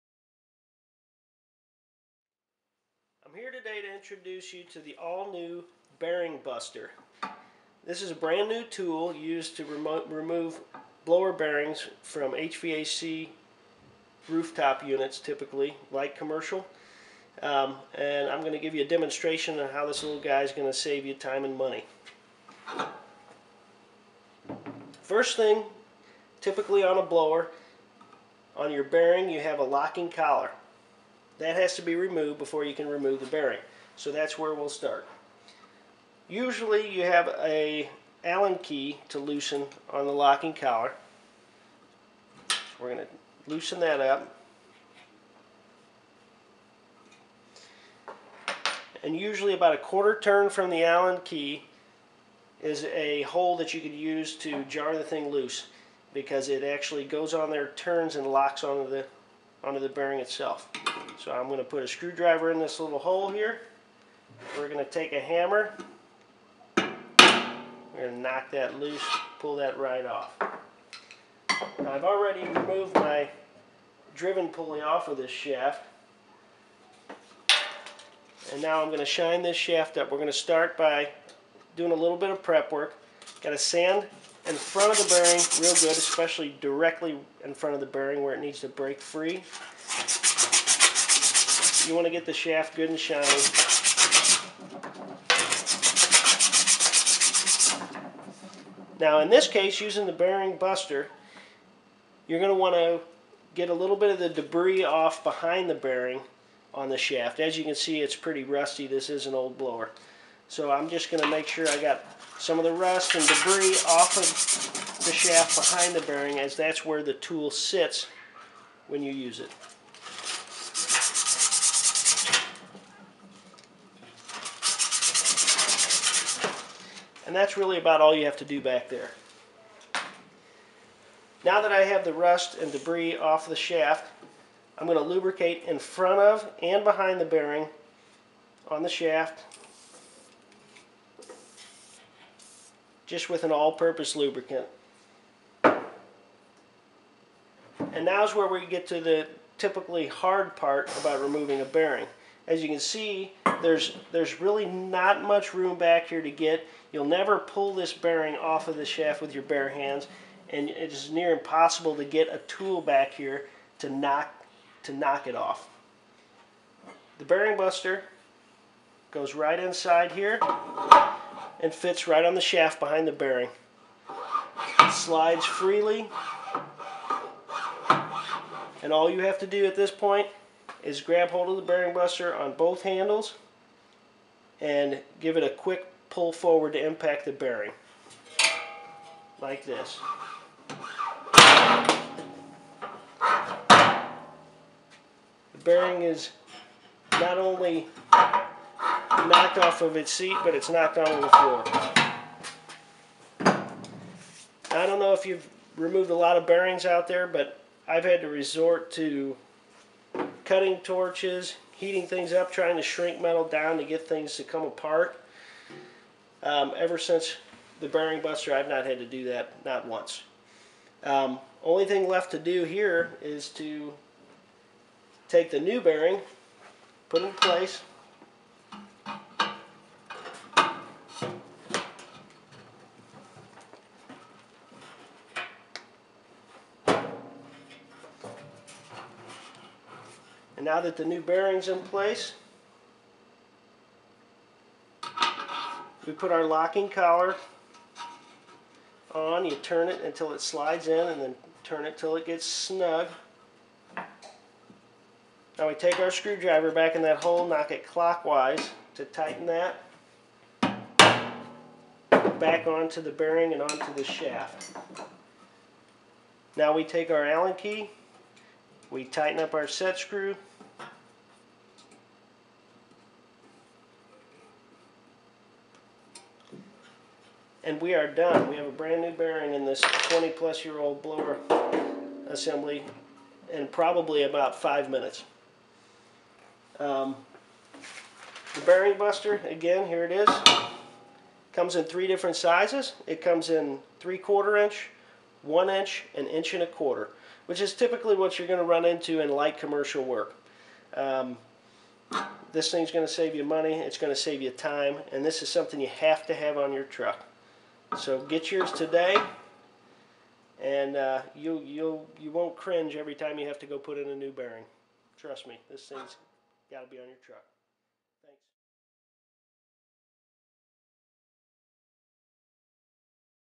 How to remove a blower bearing with the Bearing Buster tool
Bearing Buster is a revolutionary tool that makes removing old rusty blower bearings a simple task.  This video demonstrates how to remove a rusty blower bearing in under 4 minutes.